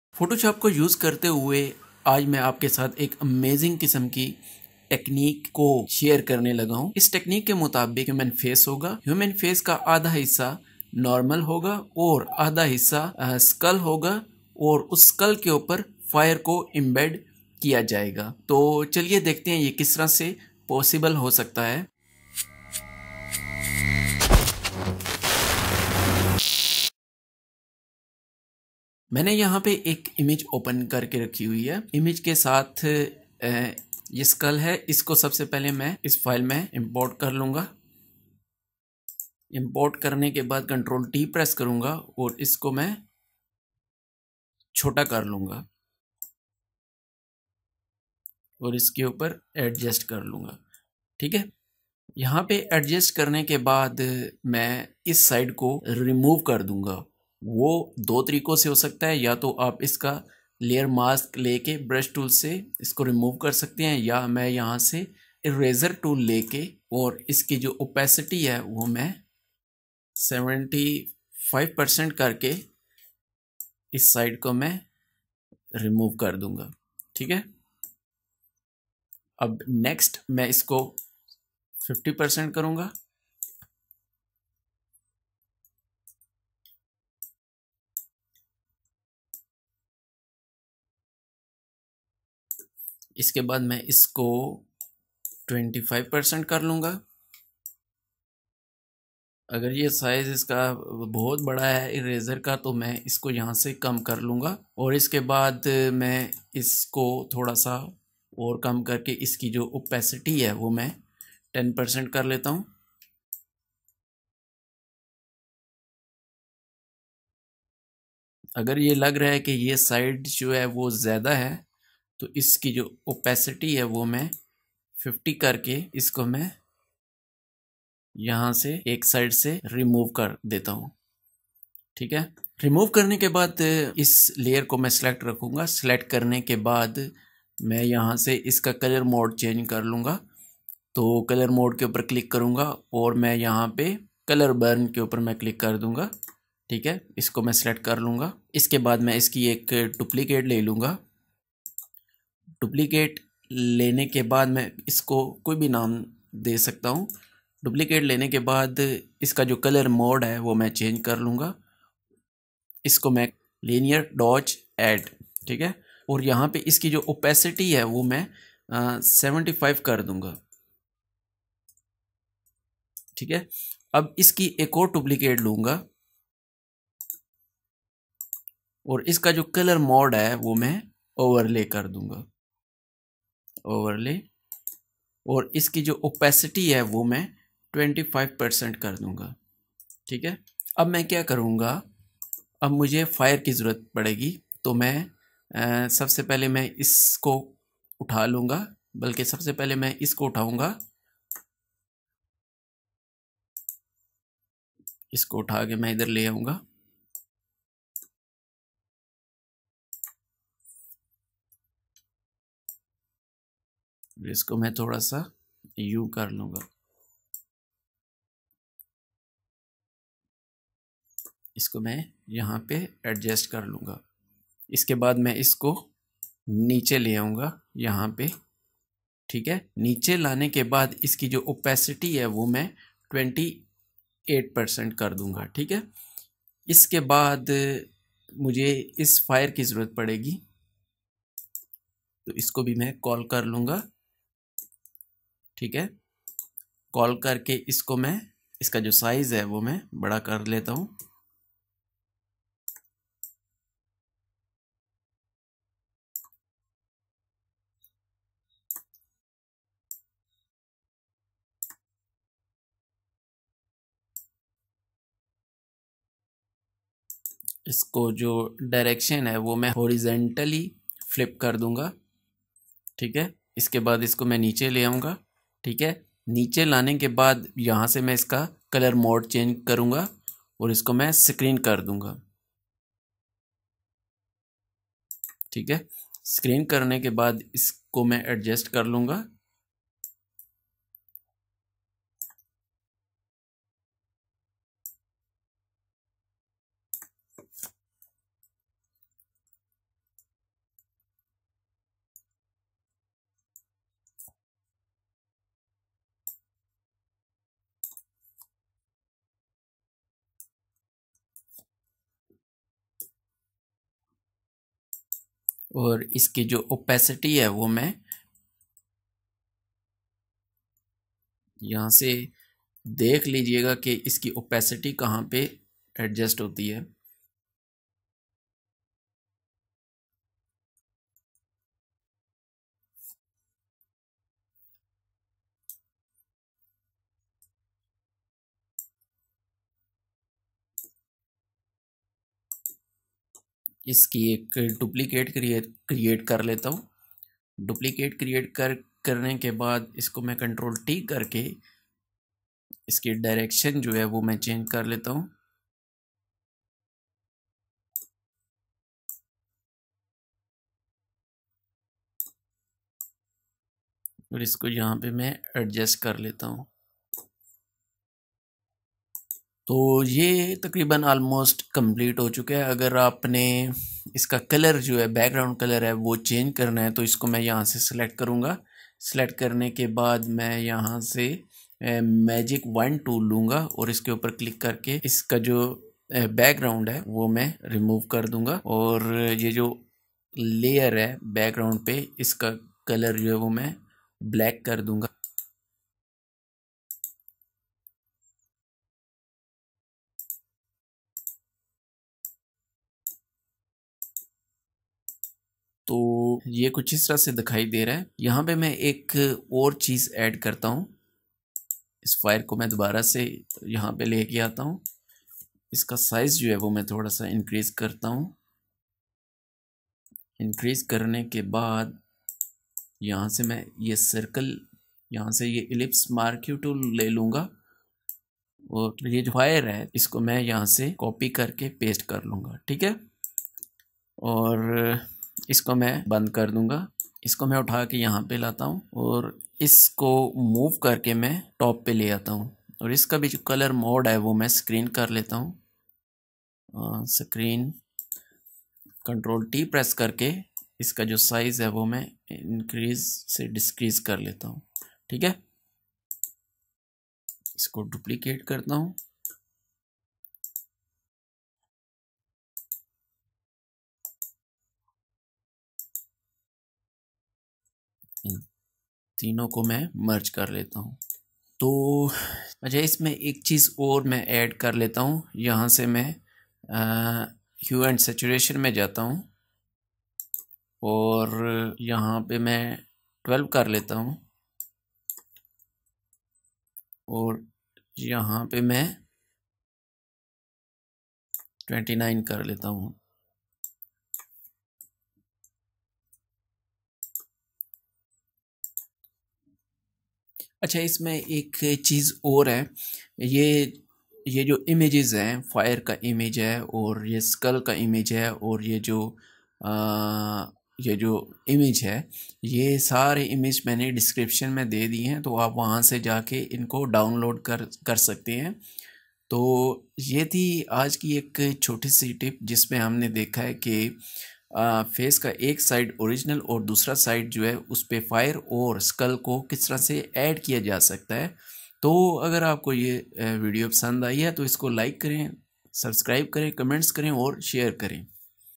Skull with Fire Effect on Face using Photoshop Urdu/Hindi
In this tutorial I will show you an amazing technique using adobe photoshop. I will show you how to merge or embed skull and fire on human face using adobe photoshop. it's not difficult its very easy just follow some setups.